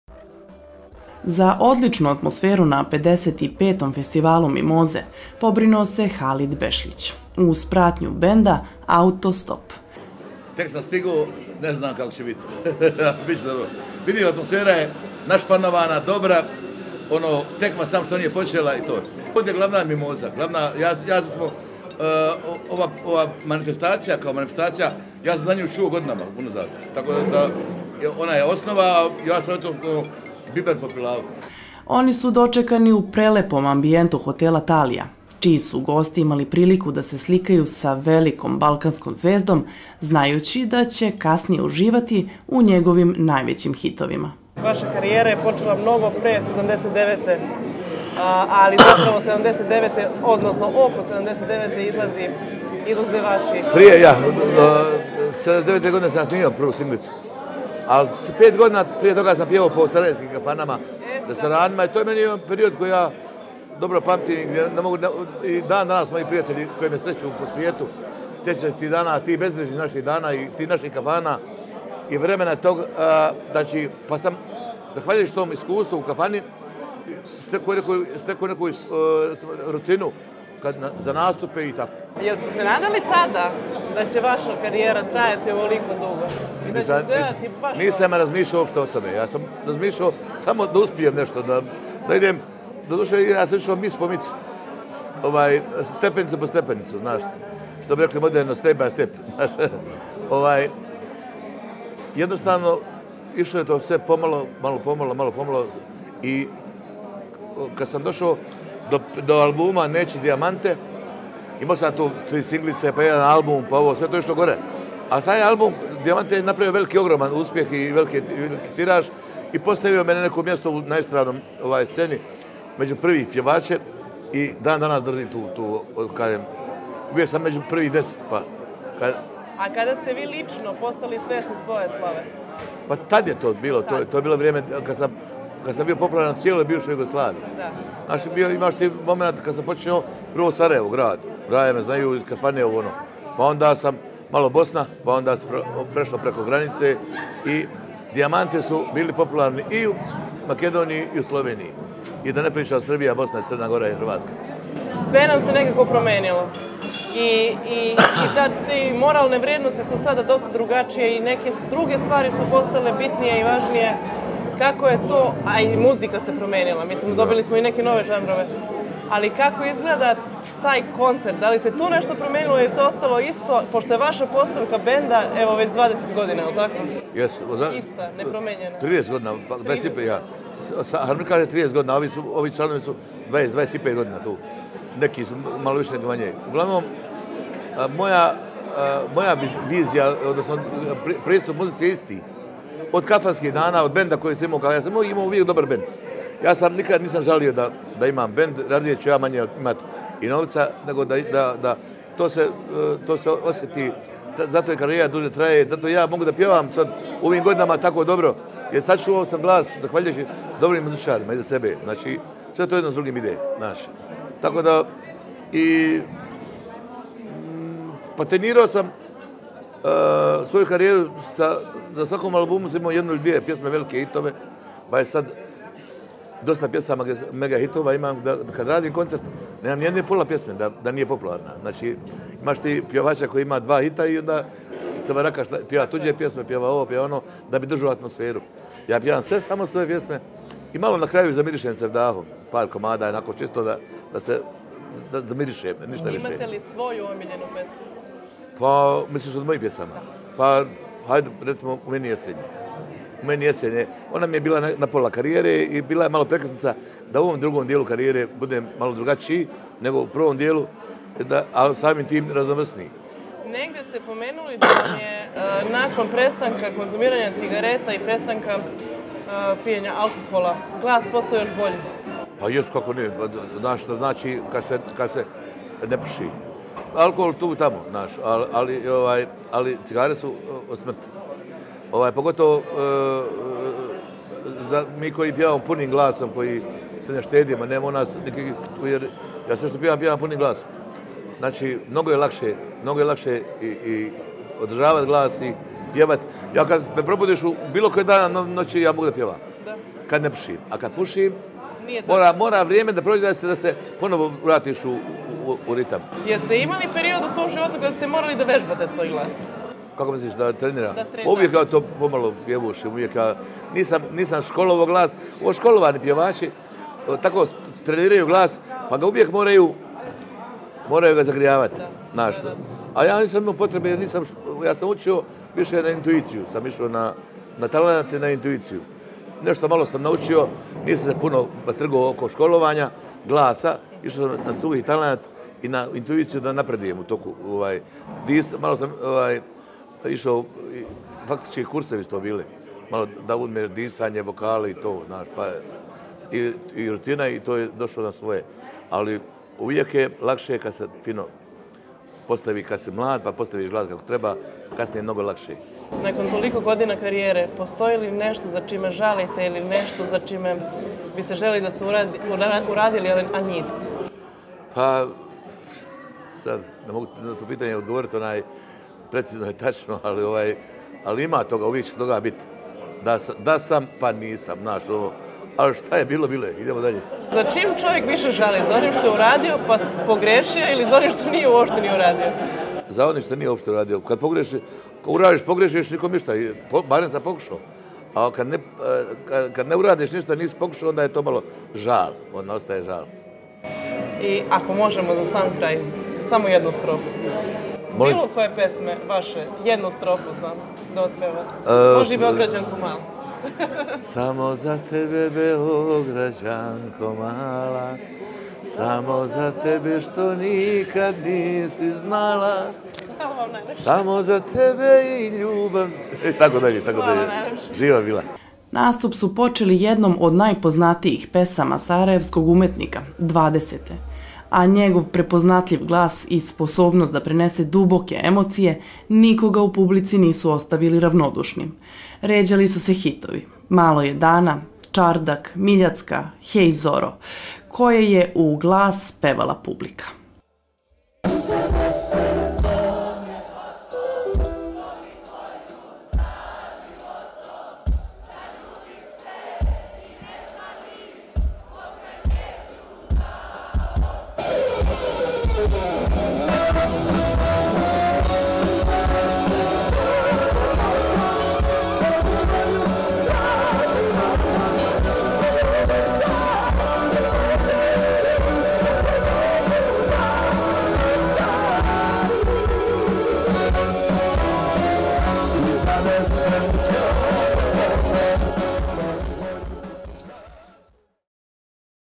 Intervju sa Halidom Bešlićem ; Preko 25.000 ljudi uživalo na koncertu Halida Bešlića u Herceg Novom
Najpoznatiji bosanskohercegovački pjevač i legenda narodne muzike, Halid Bešlić, umjetnik je čija popularnost ne jenjava, a njegove pjesme su jednako uspješne kod starije, kao i kod mlađe publike. Preko 25.000 posjetilaca praznika mimoze, imalo je priliku da uživa na Halidovom  koncertu, uz neke od njegovih najljepših pjesama.